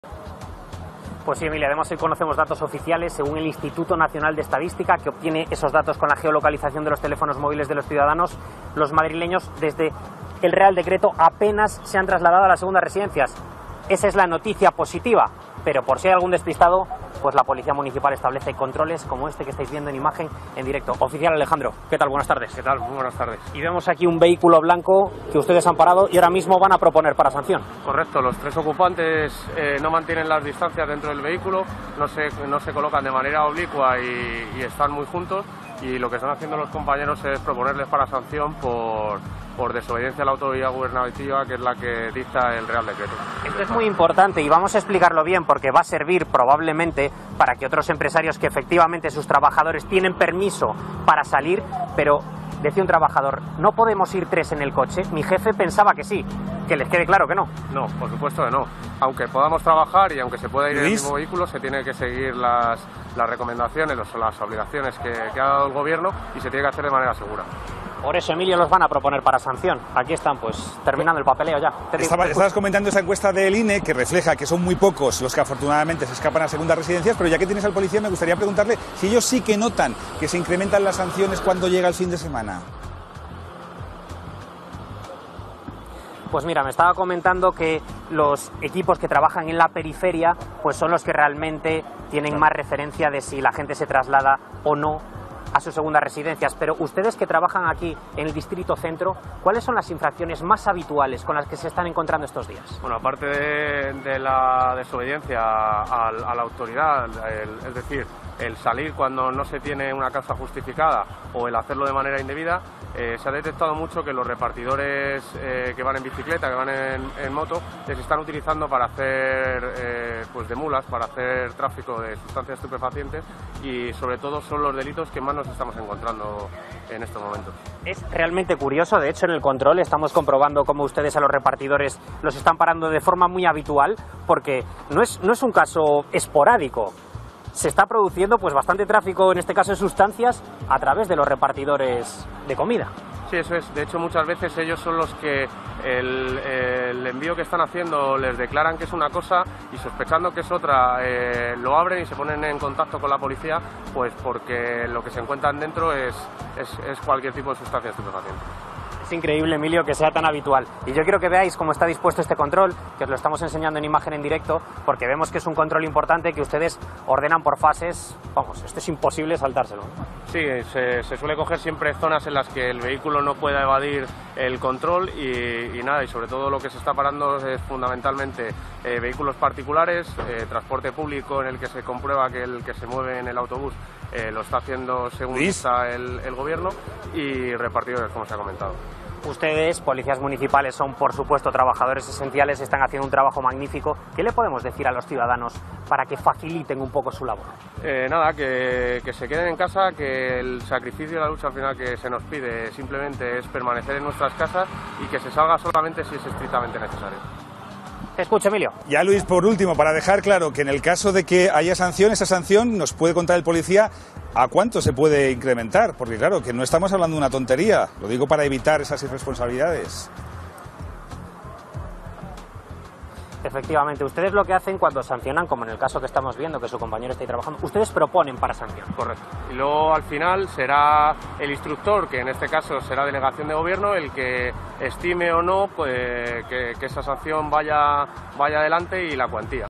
Operación "No salida" de la Policía Municipal Madrid
Arrancamos el programa en directo asistiendo a uno de los controles de la Policía Municipal de Madrid para evitar que se produzcan escapadas prohibidas este fin de semana.